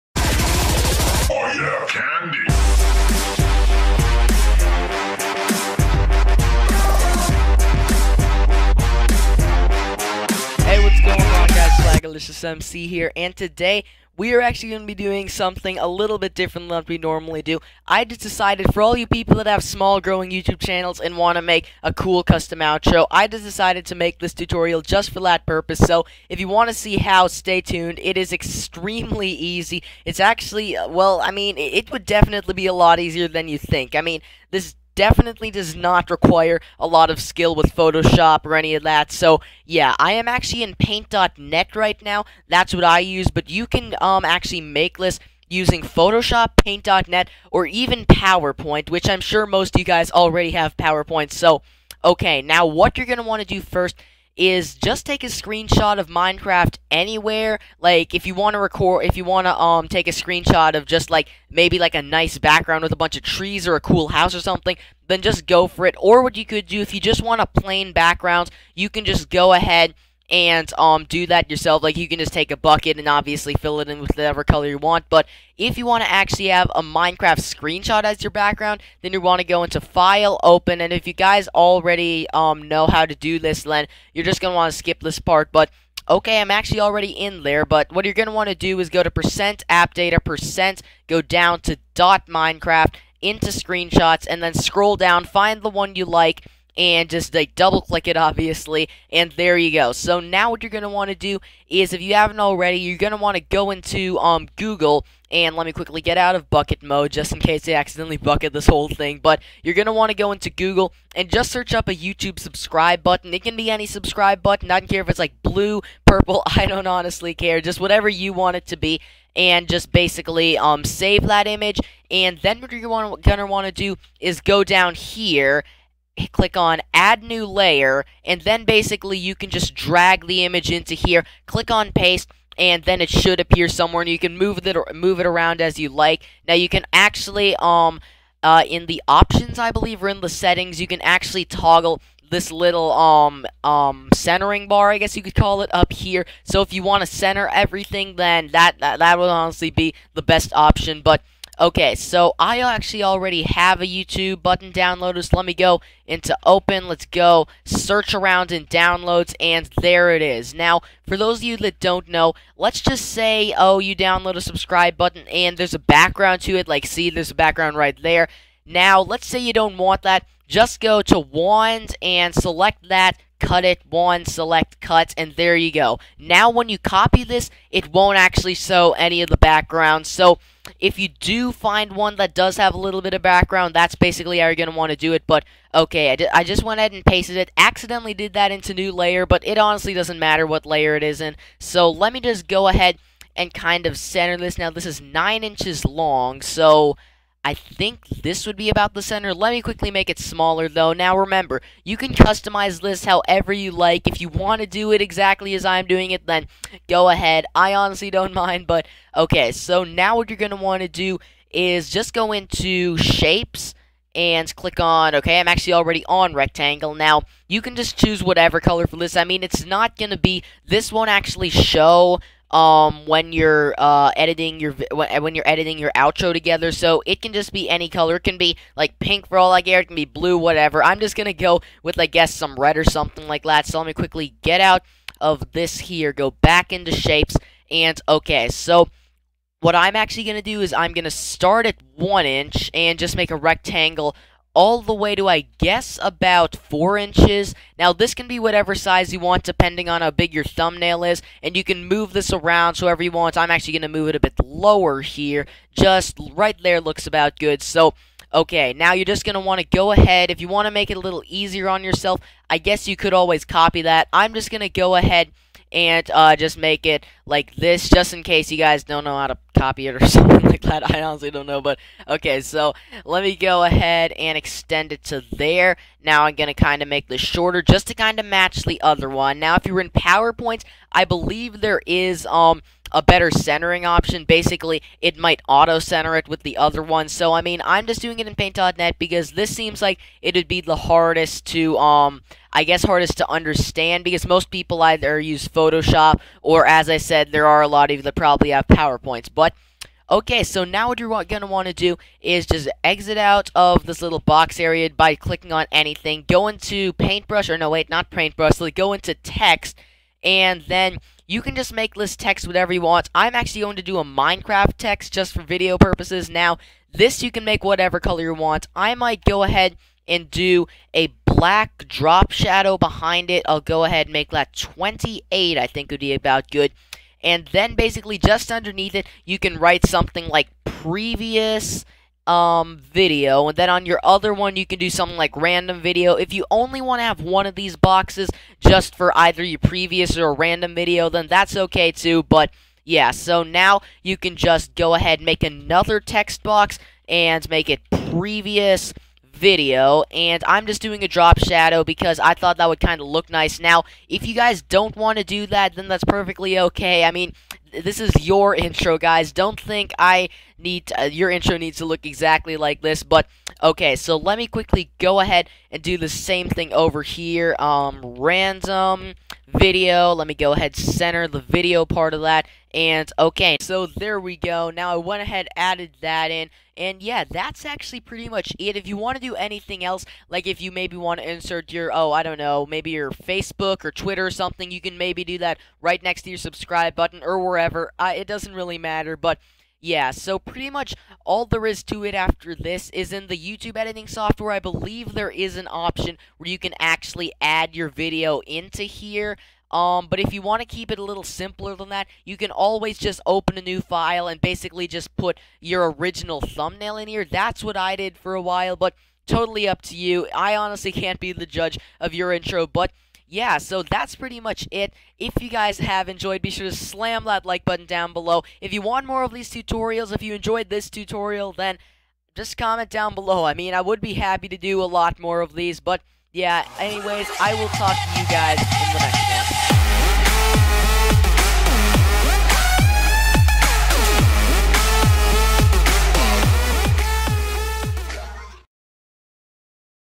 How To Make Your Own Custom Minecraft Outro!
This can be done in either powerpoint, paint.net, or photshop. Hope you guys enjoyed this little minecraft tutorial and if you wanna see more of these then SLAM that like button! :P